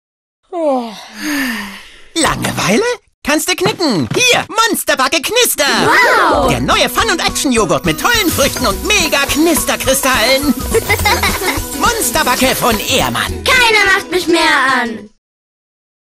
TV-Spot „Monster Backe" für Ehrmann von Heye München
Heye inszeniert für Ehrmann den neuen MONSTER BACKE Knister-Joghurt

Credits

Kunde: Ehrmann AG
Gunther Wanner (Marketingleiter)
Tanja Westkämper (Produktmanager)

Agentur: Heye München Kreation: Sven Rehfeld
Katharina Kowalski
Julia Keim
Sonja Neubert
Beratung & Strategie: Kirsten Schellberg
Sonja Mittmann
FFF: Cordula Schrenk
Produktion: Animationsfabrik GmbH (Hamburg)
Vertonung: Exit Studios
Mediaagentur: MEC